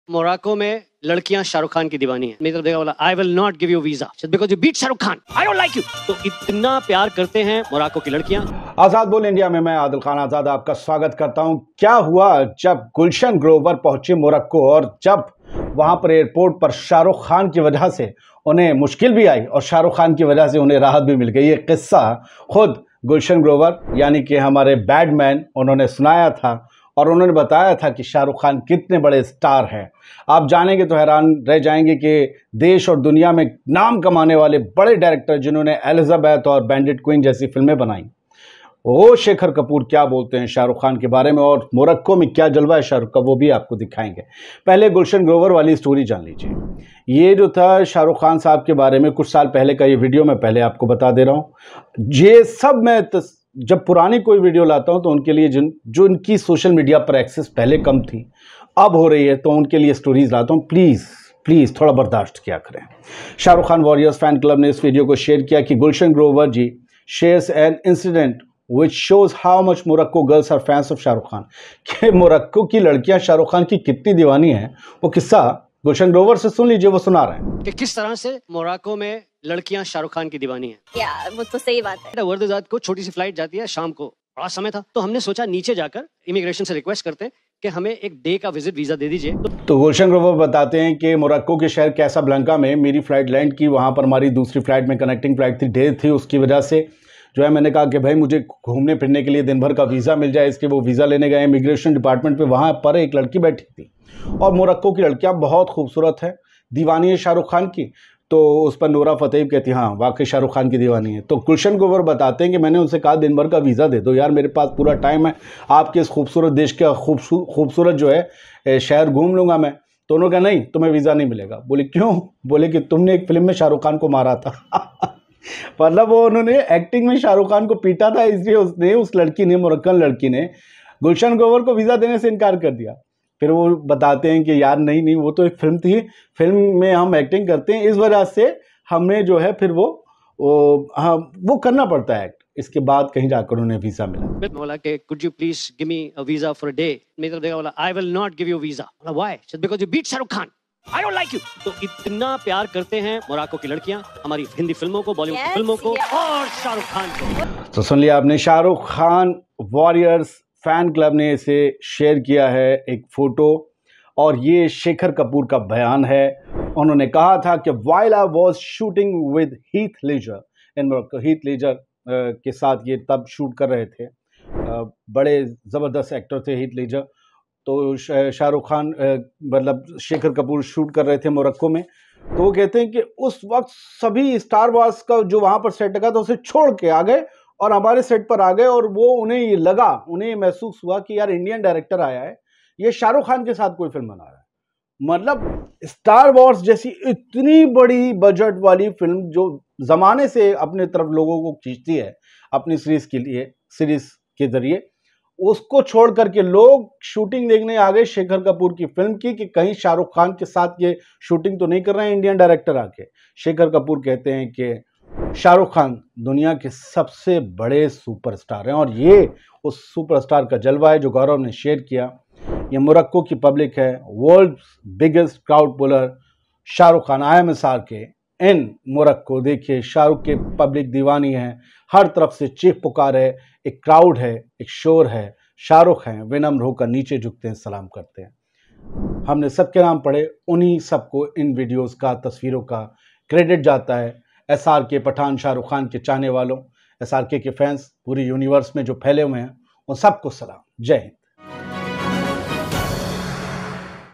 Shahrukh Khan को पीटकर अरब देश Morocco में फंस गए थे Gulshan Grover | Gulshan Grover on srk
Shahrukh Khan को पीटकर अरब देश Morocco में फंस गए थे Gulshan Grover
Gulshan Grover on srk